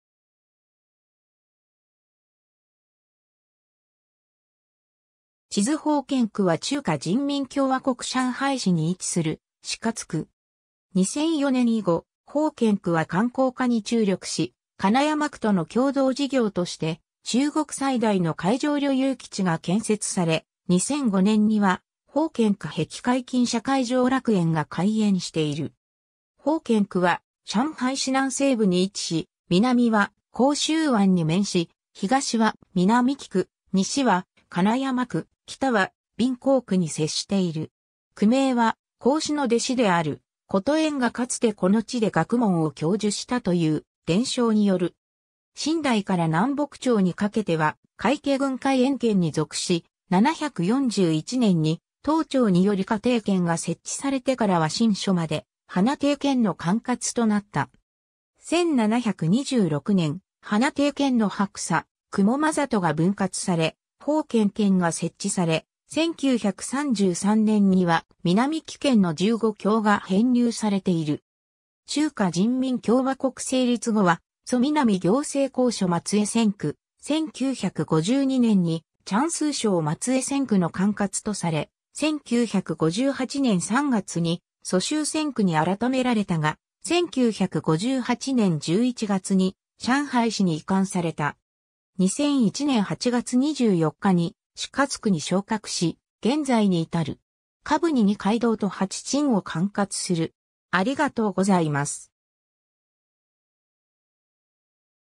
奉賢区
地図 奉賢区（ほうけんく、簡体字中国語: 奉贤区、拼音: Fèngxián Qū、英語: Fengxian District）は中華人民共和国上海市に位置する市轄区。
 2004年以後、奉賢区は観光化に注力し、金山区との共同事業として中国最大の海上旅遊基地が建設され、2005年には奉賢区碧海金沙海上楽園が開園している。
 奉賢区は上海市南西部に位置し、南は杭州湾に面し、東は南匯区、西は金山区、北は閔行区に接している。
 区名は孔子の弟子である言偃がかつてこの地で学問を教授したという伝承による。
 秦代から南北朝にかけては会稽郡海塩県に属し、741年（天宝10年）に唐朝により華亭県が設置されてからは清初まで華亭県の管轄となった。
1726年（雍正4年）、華亭県の白沙・雲間郷が分割され奉賢県が設置され、1933年には南匯県の15郷が編入されている。
 中華人民共和国成立後は蘇南行政公署松江専区、1952年に江蘇省松江専区の管轄とされ、1958年3月に蘇州専区に改められたが、1958年11月に上海市に移管された。
2001年8月24日に市轄区に昇格し現在に至る。
 下部に2街道と8鎮を管轄する。